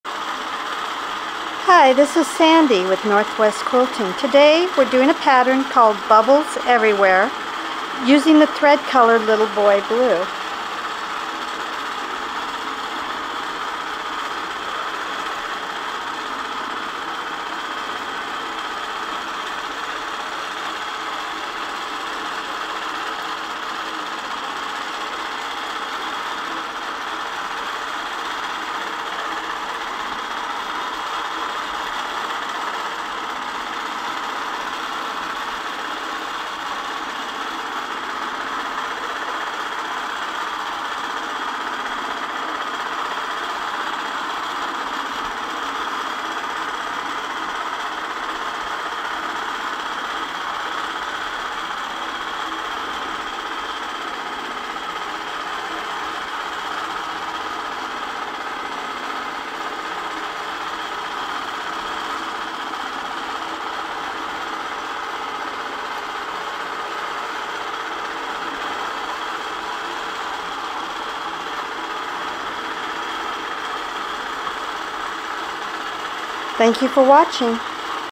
Bubbles Everywhere 10 Quilt Stitching
Video of Longarm Quilt Stitching filmed at NW Quilting Service
Over 1000 stitching patterns available to choose from!  Visit our YouTube channel